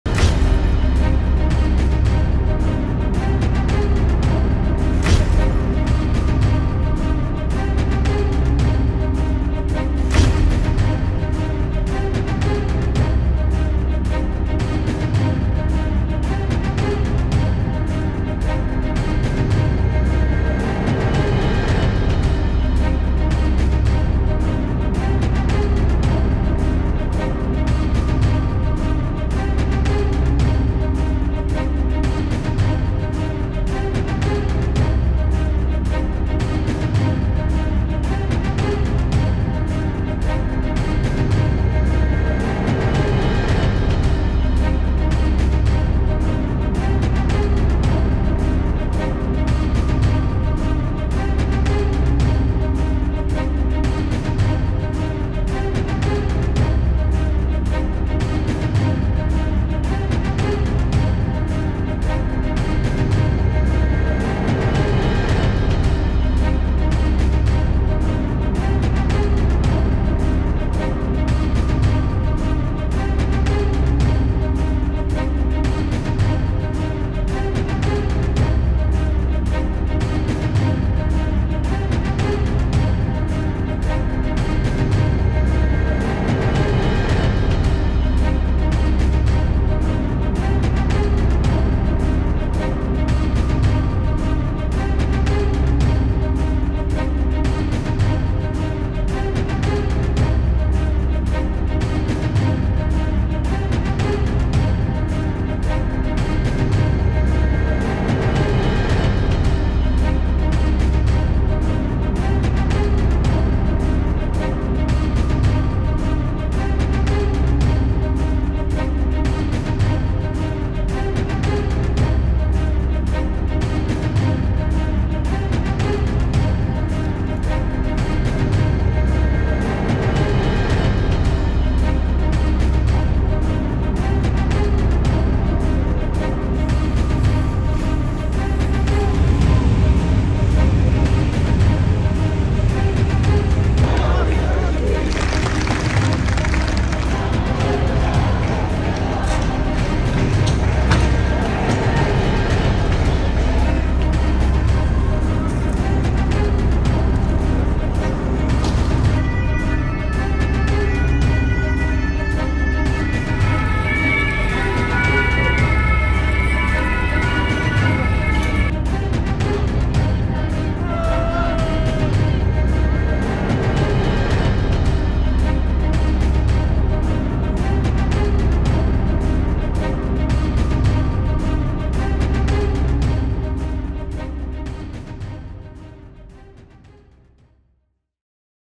France burns as Macron Govt survives no-confidence vote; Pension bill stir intensifies | Watch
The Emmanuel Macron government narrowly survived a vote of no-confidence which was triggered over the controversial pension reform. Had the no-confidence vote been successful, Macron would have had to name a new government or call a new election. Now, the controversial bill to raise the retirement age will become a law. The no-confidence vote was held after French PM Elizabeth Borne used special constitutional power to push the bill through without a vote last week. Meanwhile, protests have escalated across France and the capital city Paris remains on the edge.